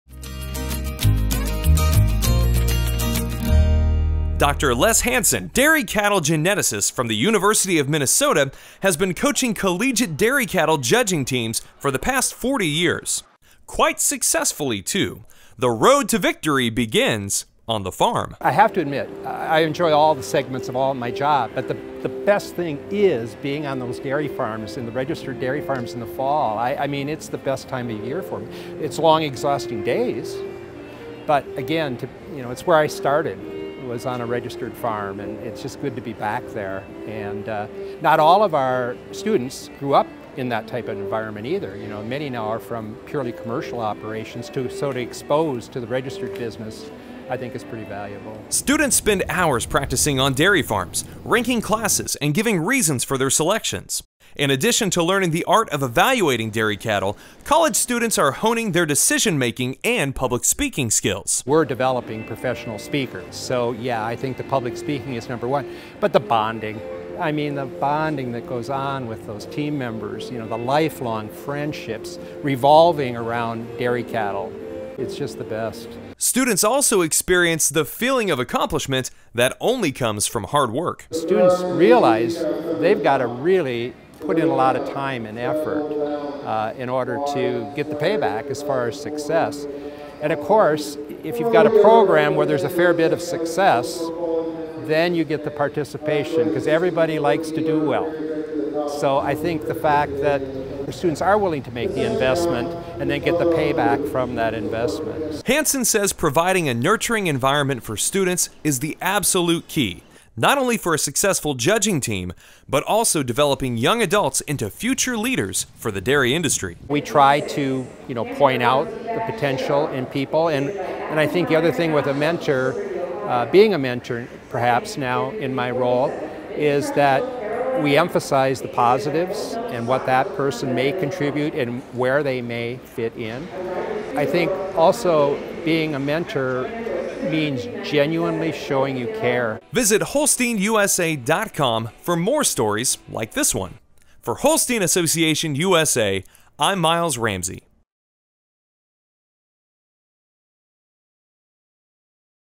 What's Up at Holstein USA: Dairy Cattle Judging
Each fall, college students put their dairy cattle evaluation skills to the test during dairy judging competitions. In this segment, Dr. Les Hansen of the University of Minnesota shares how judging teams help shape the next generation of dairy leaders.